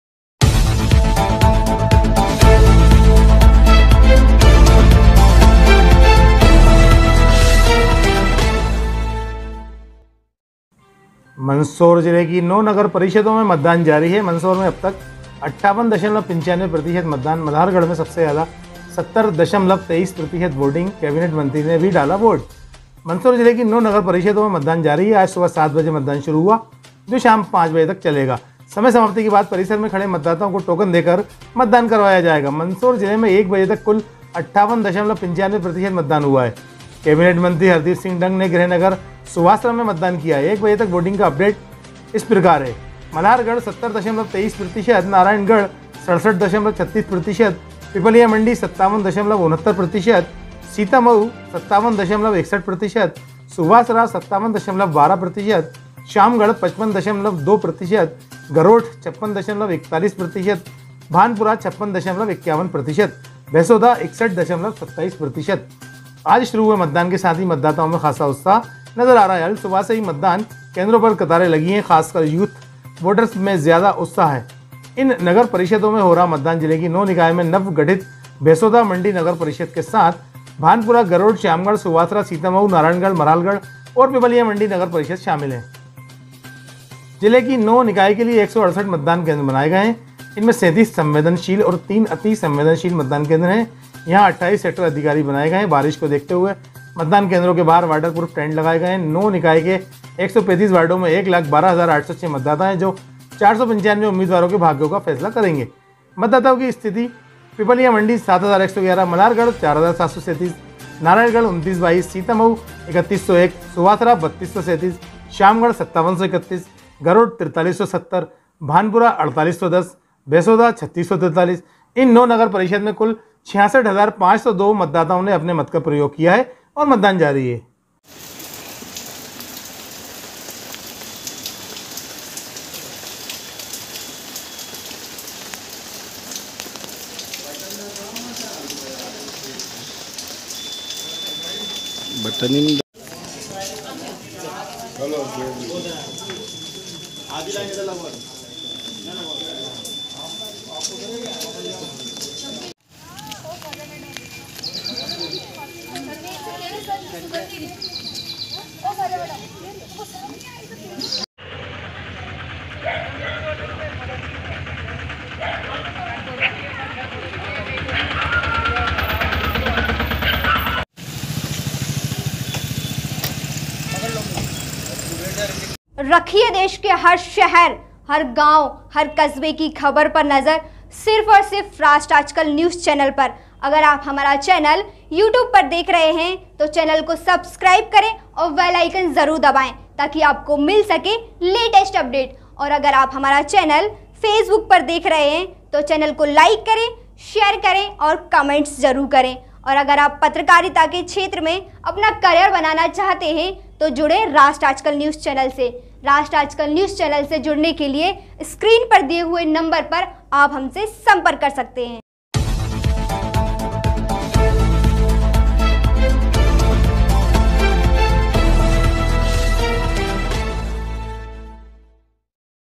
9 नगर परिषदों में मतदान जारी कैबिनेट मंत्री ने भी डाला वोट
9 नगर परिषदों में मतदान जारी कैबिनेट मंत्री ने भी डाला वोट